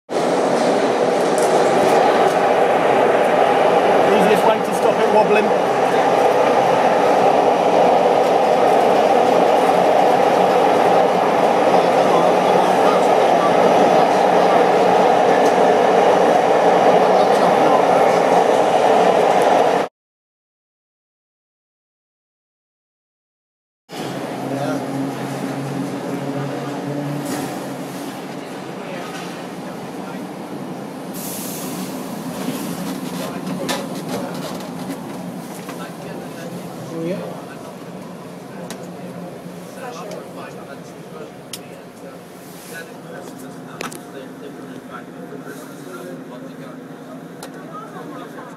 Heritage Train to Heathrow - View from the rear.
Leaving Terminals 1,2,3 and entering Terminal 5.